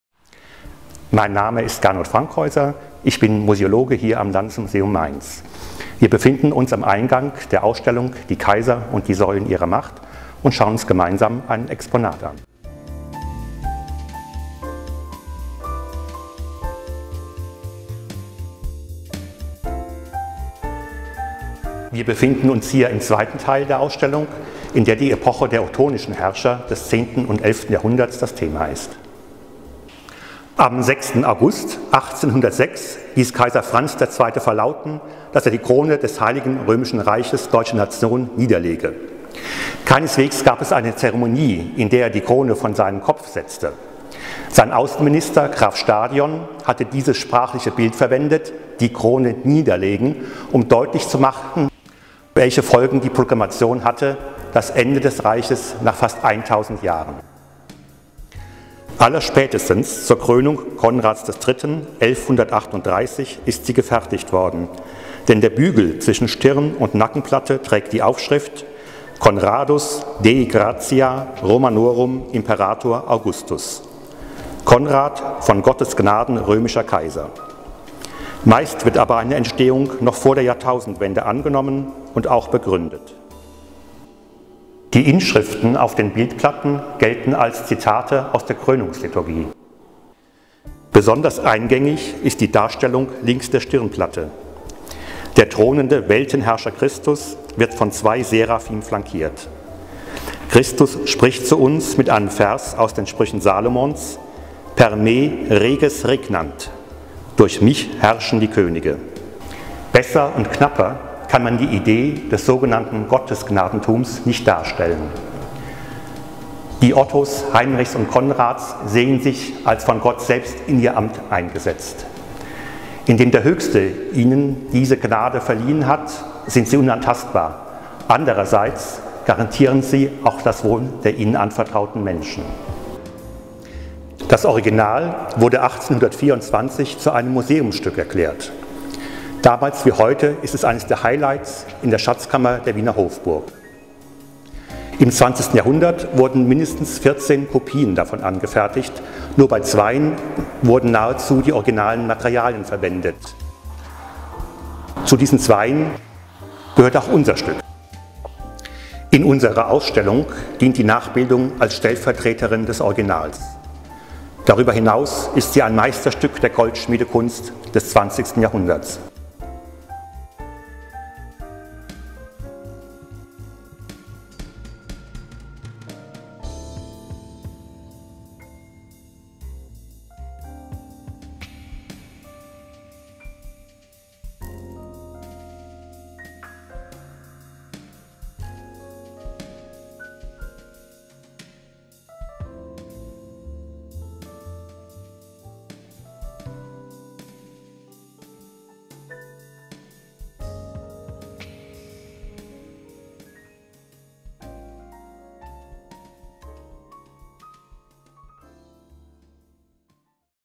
Digitale Kurzführung: Gernot Frankhäuser präsentiert die Reichskrone aus Mainz
Kaiserkronen begeistern viele Museumsbesucher*innen. In dieser Folge der digitalen Kurzführung stellt Gernot Frankhäuser die Nachbildung der Reichskrone aus Mainz vor, die zwischen 1974/1975 entstanden ist und sich heute als Dauerleihgabe im Besitz des Landesmuseums Mainz befindet.